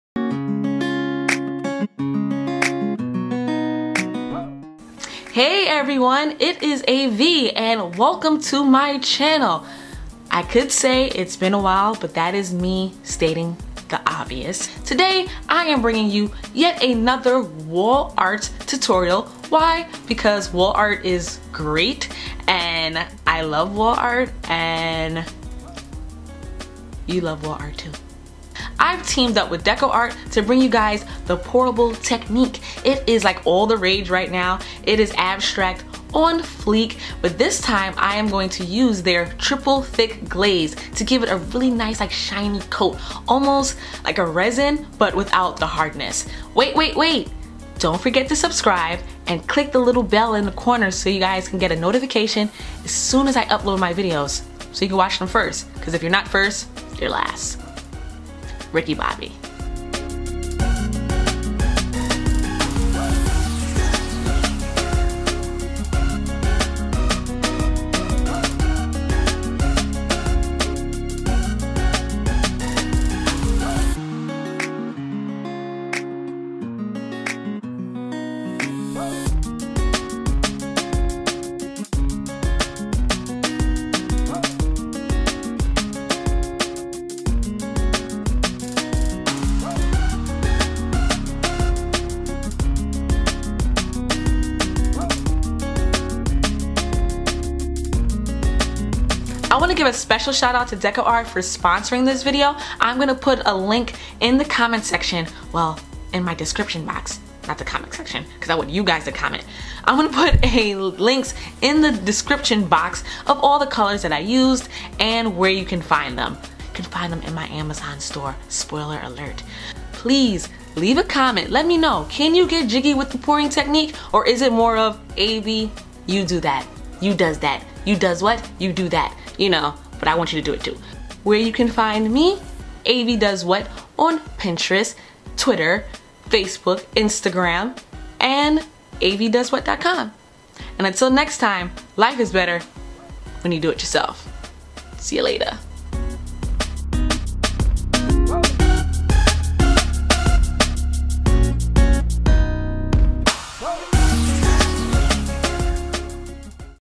Super Easy Paint Pouring on Canvas
This easy project can be done in no time and is one of a kind. I used Decoart acrylic paint, water and Decoart's Triple thick glaze to create this colorful canvas art.

Visit Decoart.com for more inspiration.

Please note that all projects seen on this channel must be completed at your own risk. I do not take responsibility for any harm or injury that may occur. Be safe!

*Note* I make a commission from your purchase but I use everything I recommend!